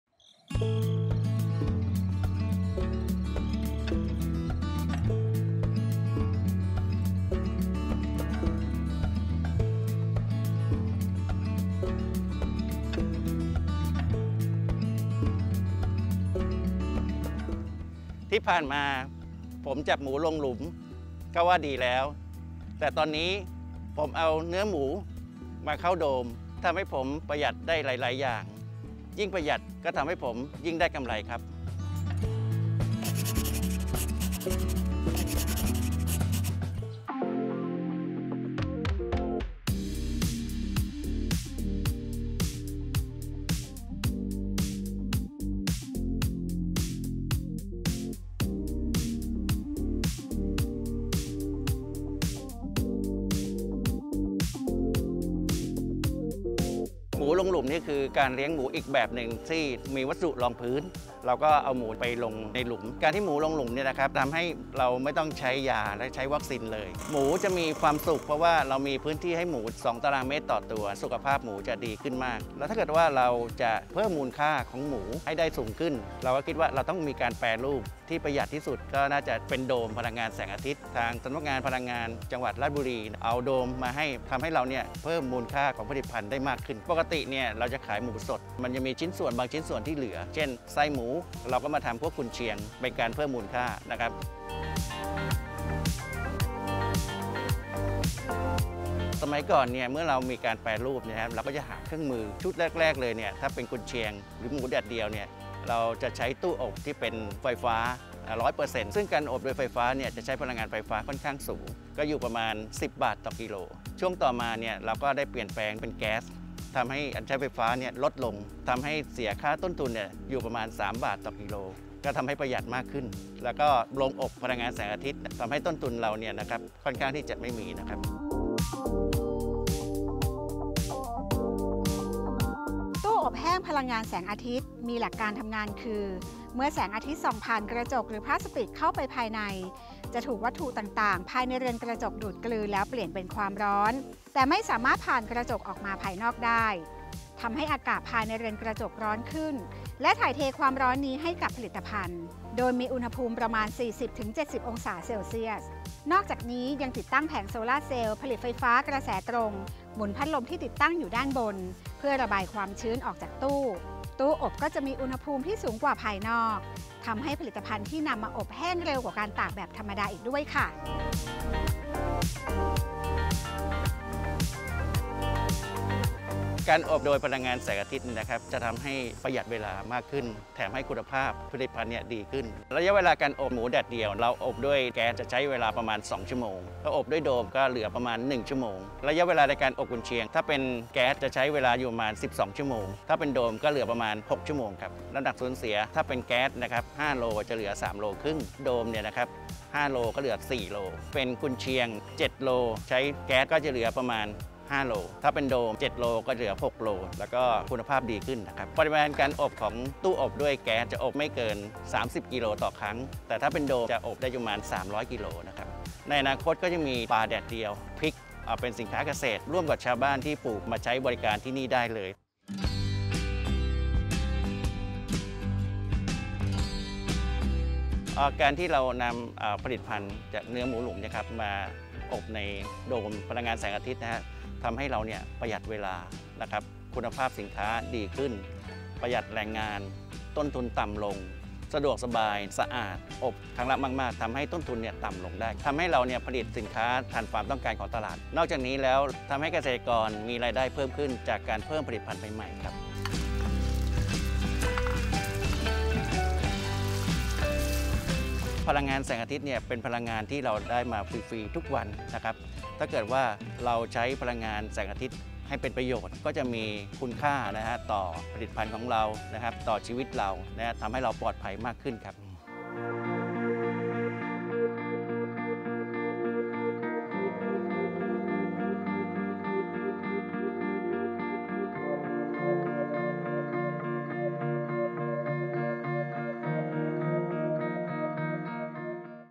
908 หมูหลุมอบแห้งด้วยพลังงานฟรี รู้ค่าพลังงาน
กลุ่มวิสาหกิจเลี้ยงหมูหลุมดอนแร่ จ.ราชบุรี มีเทคนิคในการพัฒนาและต่อยอดธุรกิจอย่างไร ถึงประหยัด ลดรายจ่าย และมีกำไรงาม ต้องชม!
ออกอากาศ  8 ก.ค. 64